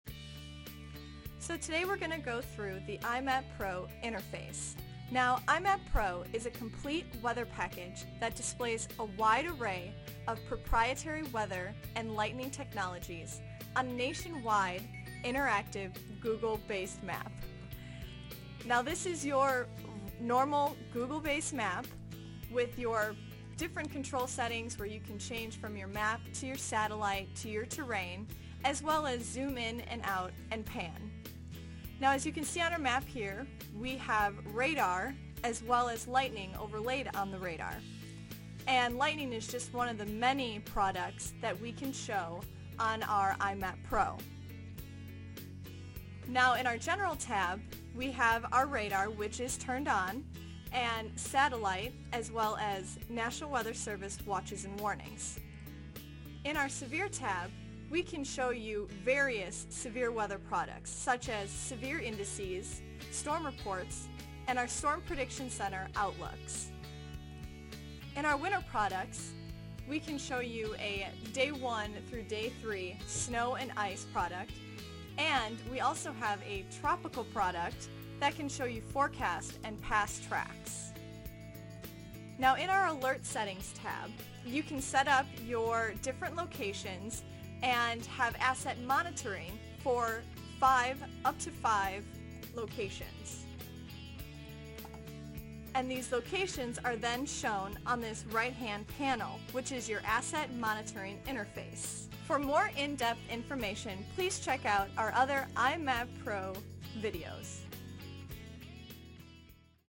iMapPro - Professional Weather Maps
The iMapPro display software offers interactive and customizable weather maps. With iMapPro, you can display all your mission critical locations and assets for real-time monitoring, and customize your display by choosing from a wide variety of severe weather layers.